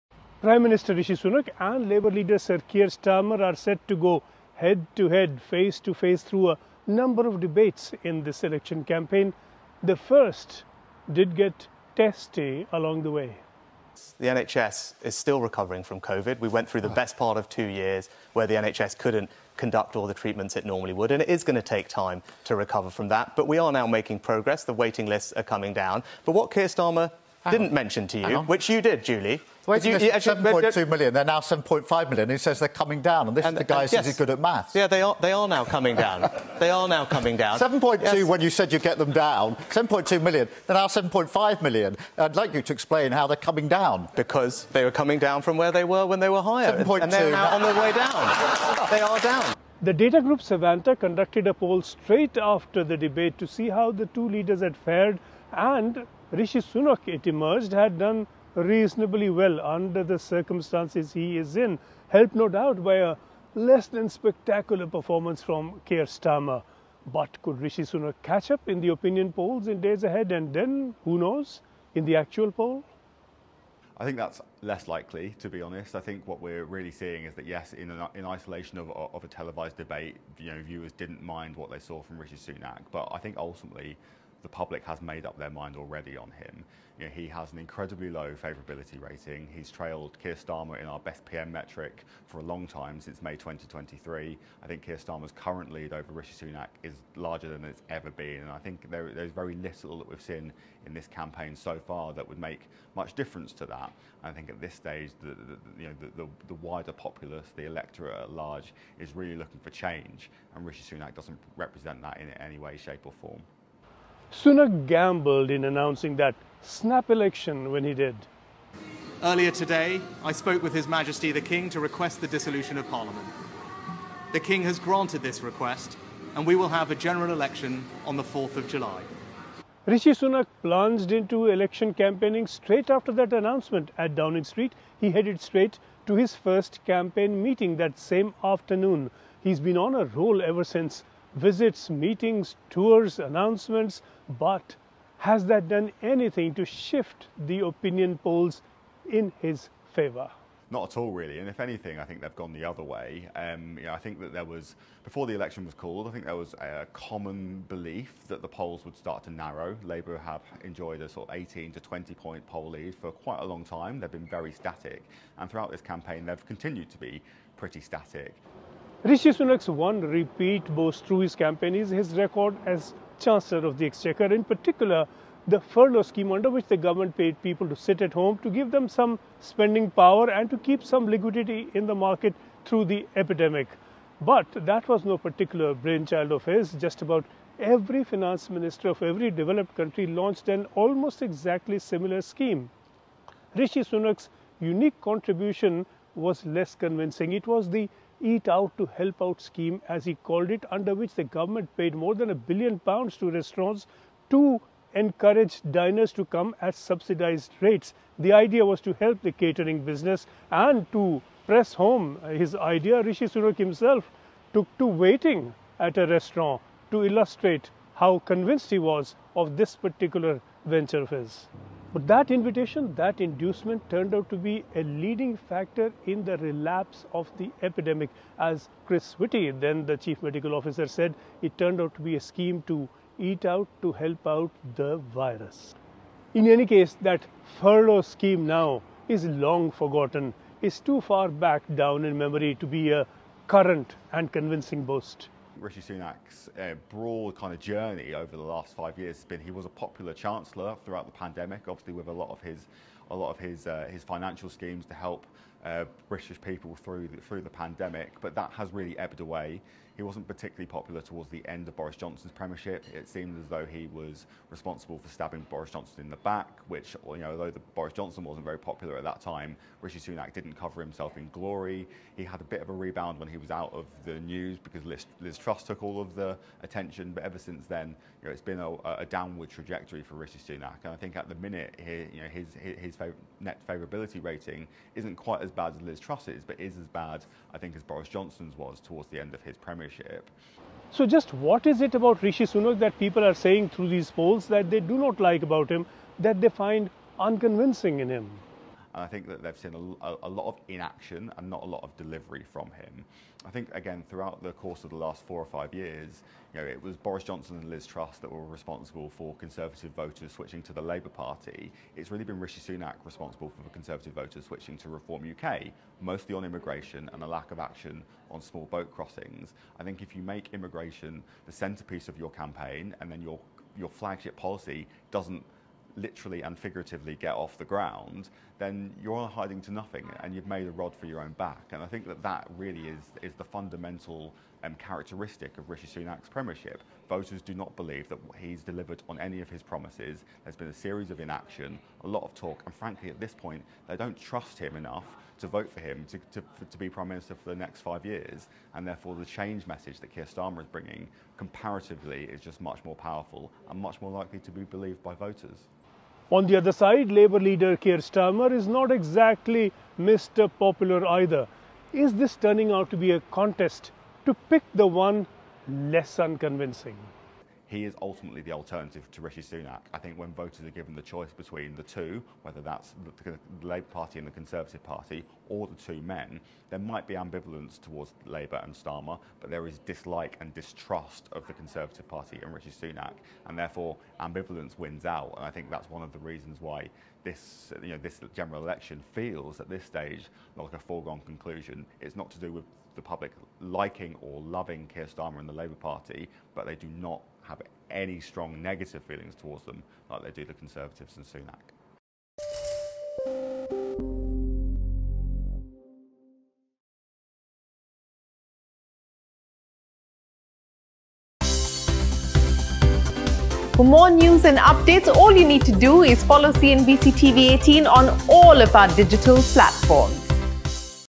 UK PM Rishi Sunak & Sir Keir Starmer Set For Showdown Ahead Of Polls | N18G
As the United Kingdom gears up for the election on July 4, PM Rishi Sunak trails behind Sir Keir Starmer on the favourability factor despite a solid debate showing. Savanta data indicates a growing lead for Starmer, reflecting the electorate's desire for change. Sanjay Suri reports

n18oc_business